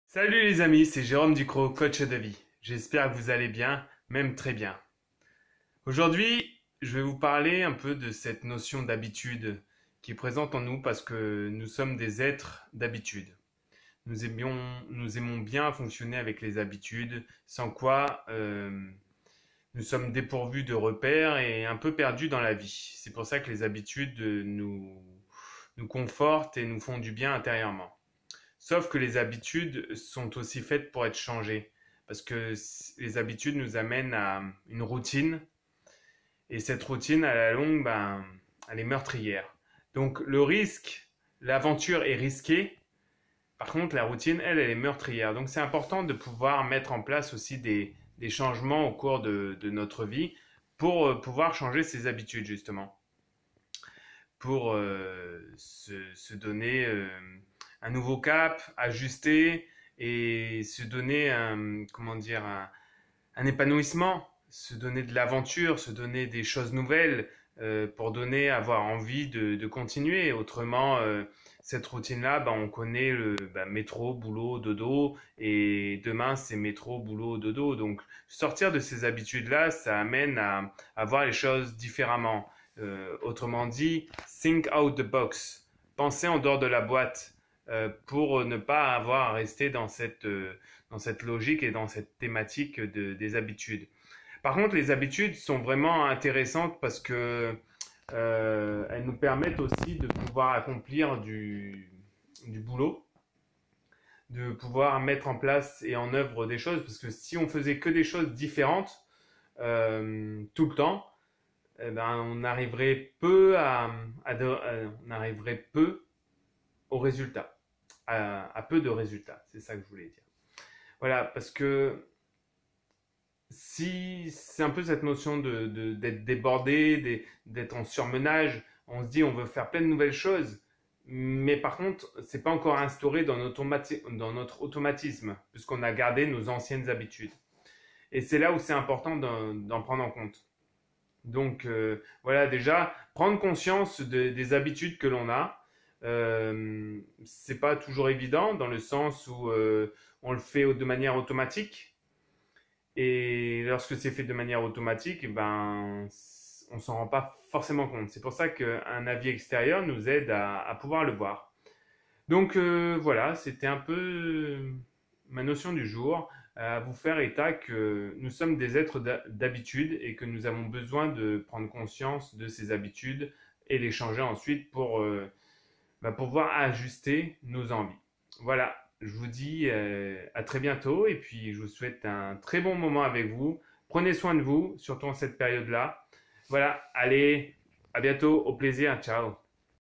Nous sommes des êtres d'habitudes.
Cela nous donne un certain confort, cependant comme il n'y a qu'une chose qui est permanente dans la vie: c'est le CHANGEMENT.
Car tout ce qui stagne fini par mourir.

Ajustons nos habitudes en y mettant de la conscience, du partage et de l'envie.

Prenez soins de vous.